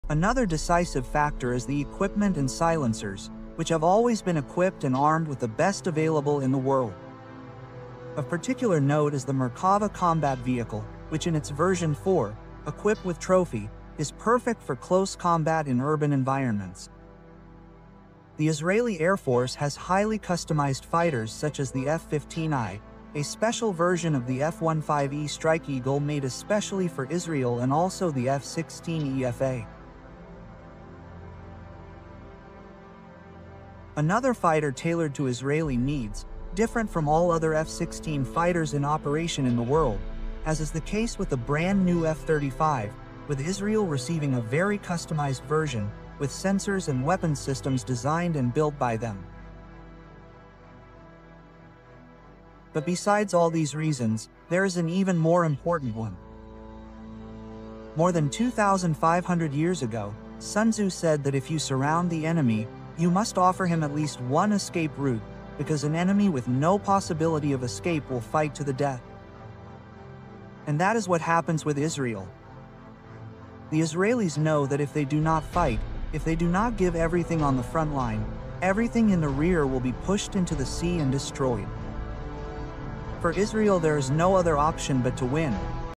Inside Israel’s High-Tech Military Gear That Dominates Every Battlefield!
Get an exclusive look at Israel’s cutting-edge military technology that gives them the upper hand in every conflict! From custom fighters to advanced tanks, this clip is a must-watch for tech fans and defense geeks. Share the future of warfare now!